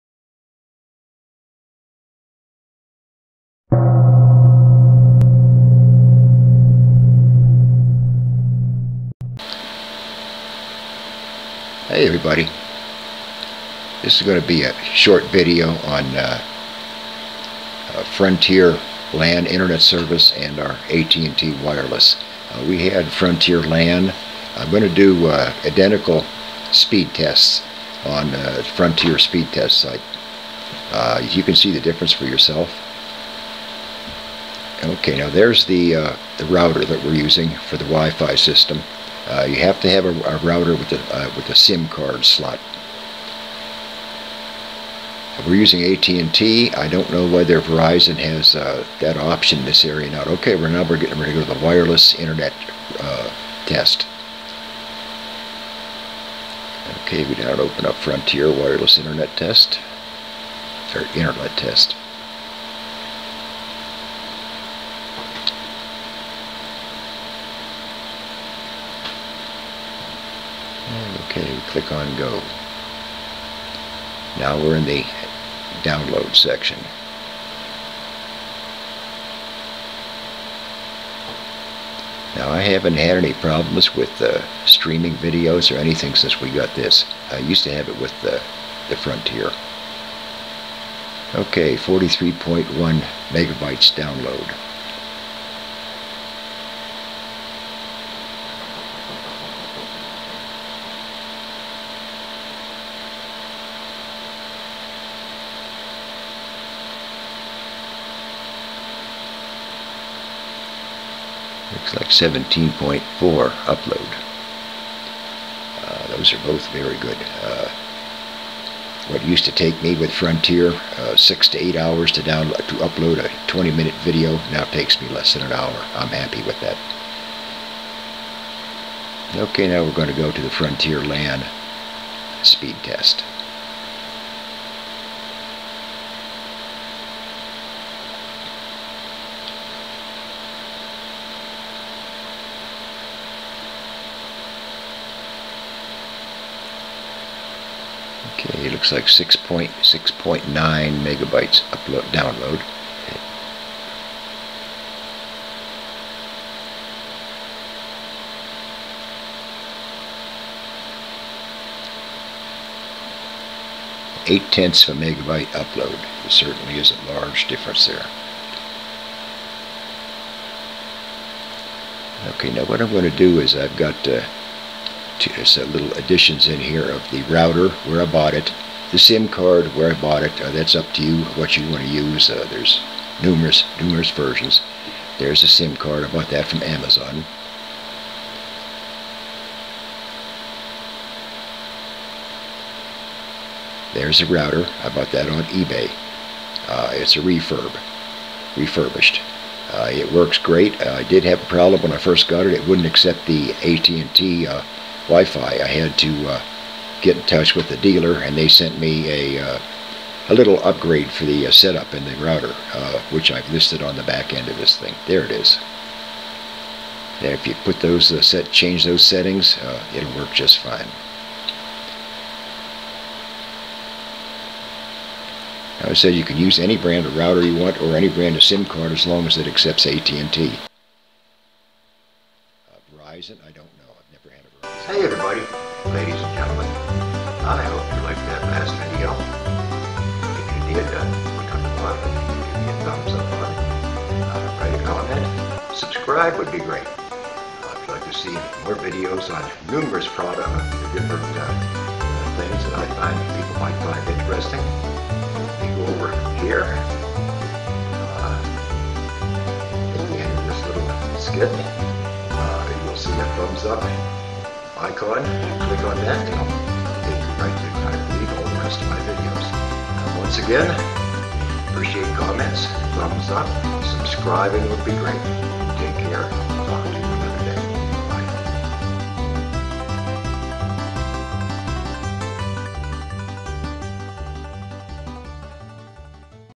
Frontier Lan, versus Wifi, A short Internet Speed Test Video .
For 7 Years we put up with Chronic Internet Problems not counting slow Speeds, Frontier just didn't seem to care when we had a problem...After doing some research I found out there were other options available to country dwellers. One being Hot Spot Wifi, if you have a Cell Phone Signal it is available to you...Here is an internet Speed Test between Frontier Lan, and AT&T Wifi. This is Frontier's Speed Test Address.